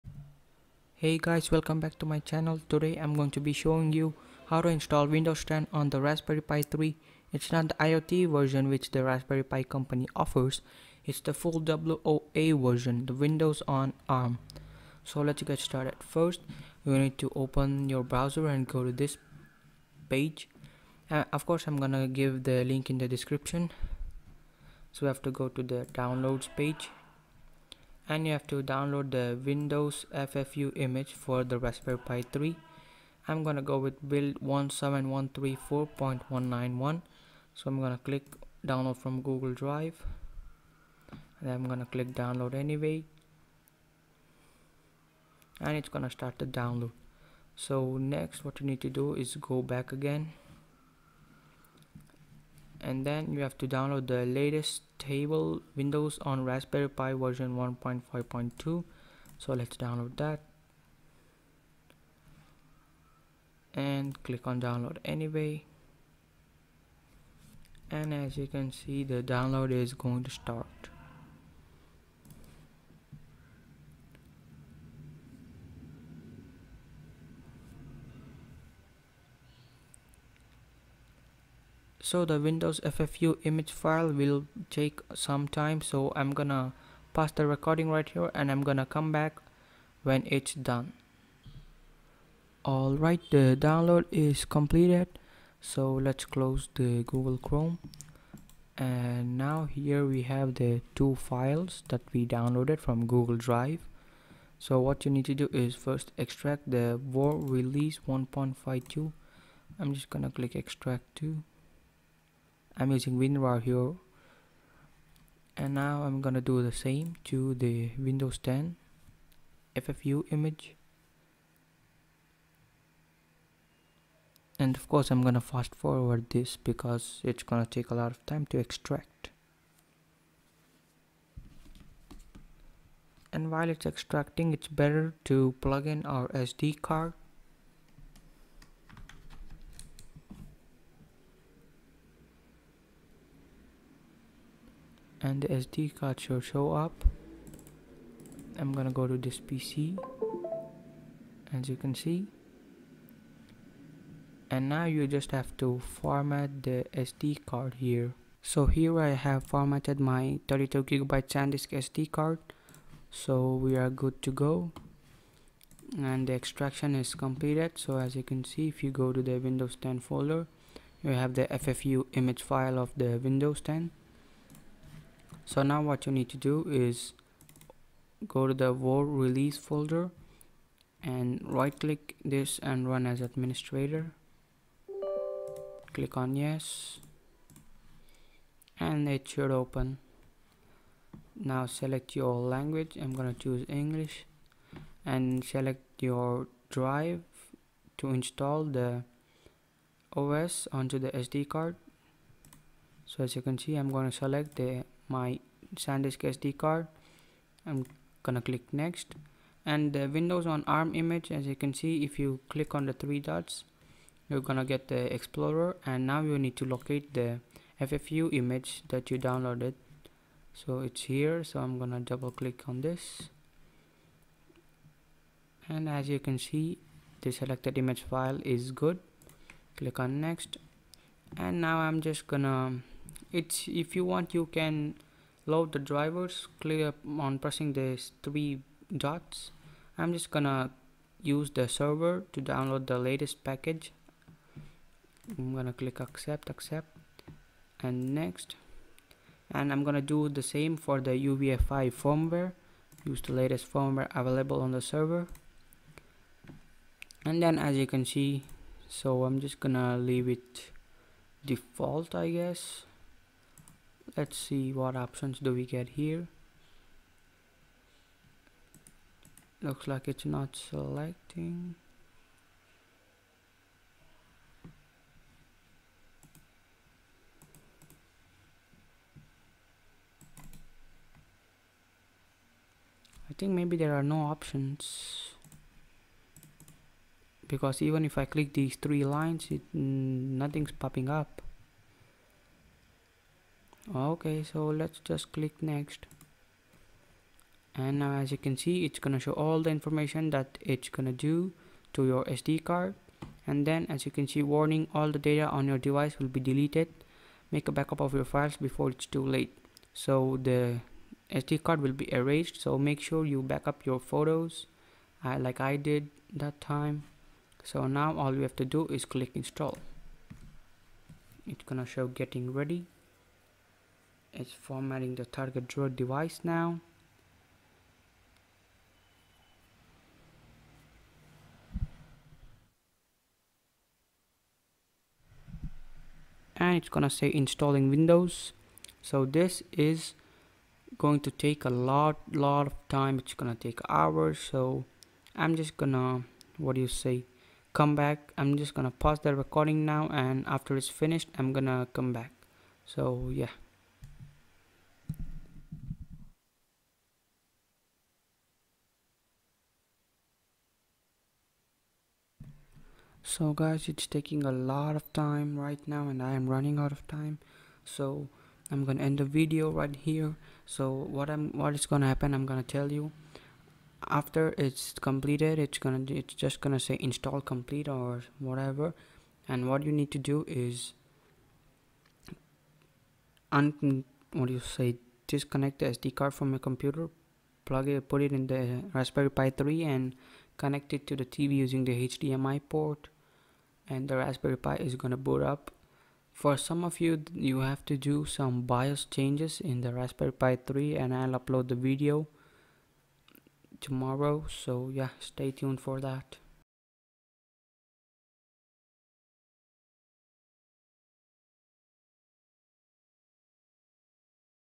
How to install Windows 10 (WOA) on Raspberry Pi 3
Hey guys this video is about installing full windows 10 on raspberry pi 3
Raspberry Pi 3
Sandisk 32GB SD Card
Laptop I Used